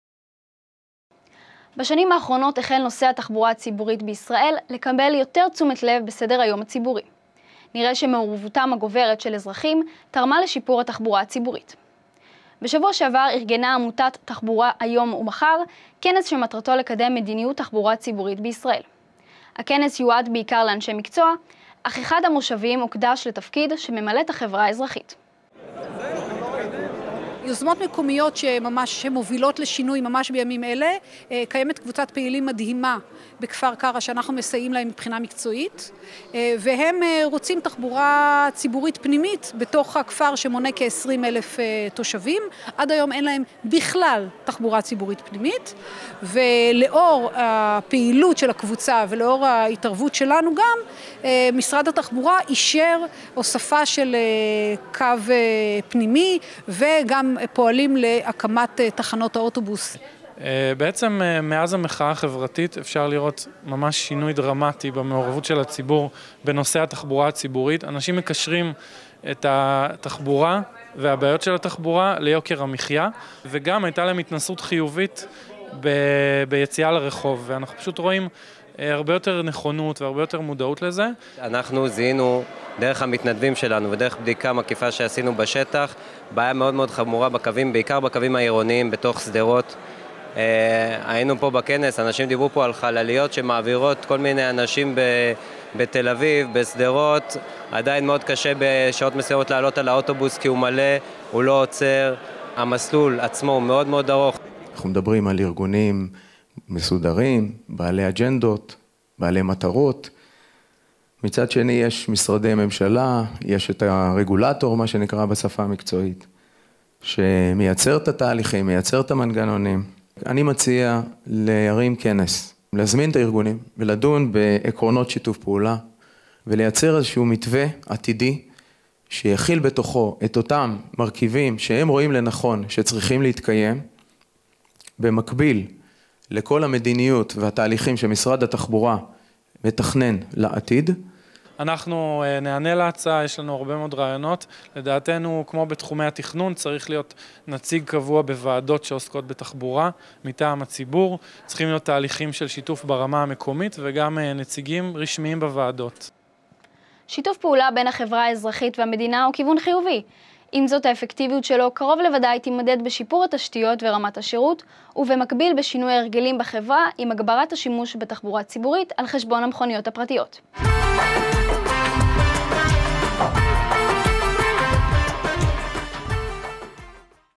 תחבורה והחברה האזרחית
בשנים האחרונות החל נושא התחבורה הציבורית בישראל לקבל יותר תשומת לב בסדר היום הציבורי. נראה שמעורבותם הגוברת של אזרחים תרמה לשיפור התחבורה הציבורית. בשבוע שעבר ארגנה עמותת "תחבורה היום ומחר" כנס שמטרתו לקדם מדיניות תחבורה ציבורית בישראל. הכנס יועד בעיקר לאנשי מקצוע, אך אחד מהמושבים הוקדש לתפקיד שממלאת החברה האזרחית.